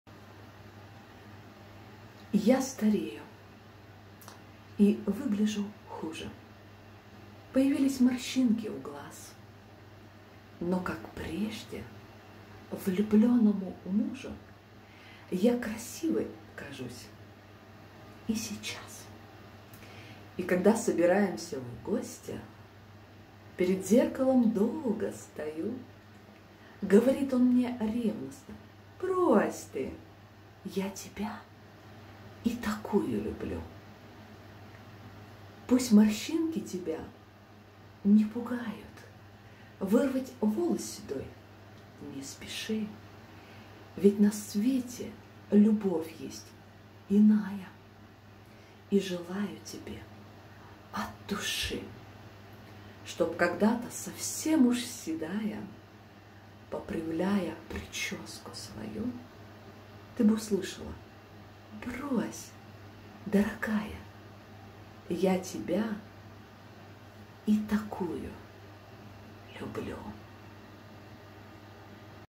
Читаю стих. "Я старею и выгляжу хуже"
Давно я не читала стихи. Сегодня прочитаю замечательный стих.